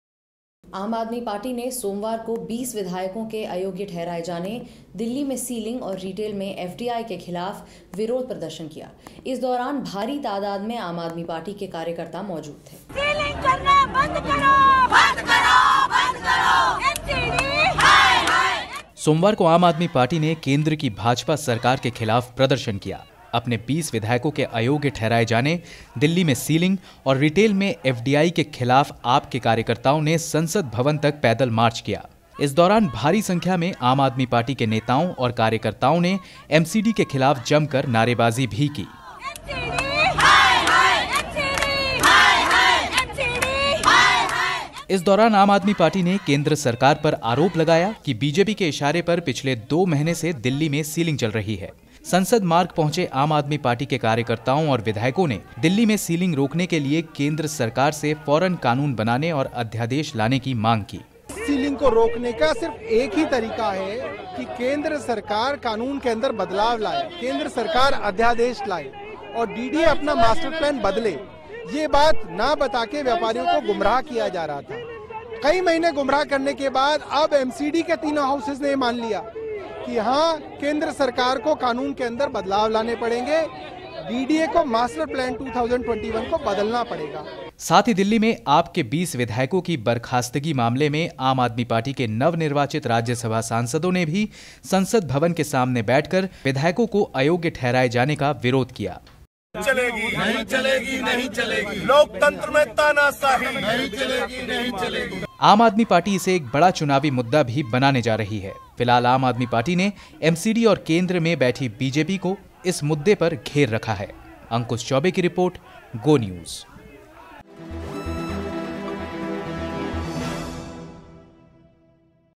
AAP Protest
AAP protests against the disqualification of its 20 MLA's, the MCD's sealing drive in Delhi and FDI in retail